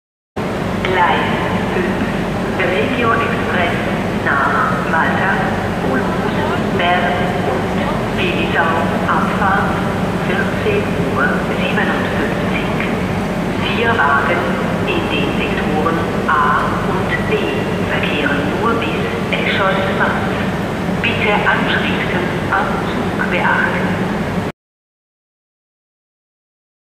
Gleis 5 RE nach Bern/Willisau in Luzern | Isabelle Augustin Stimme | Alte SBB Stimme
Ich hoffe euch hat das Video gefallen. Lasst auf jedenfall ein Abo da, vergesst nicht das Video zu Liken und fleissig zu kommentieren.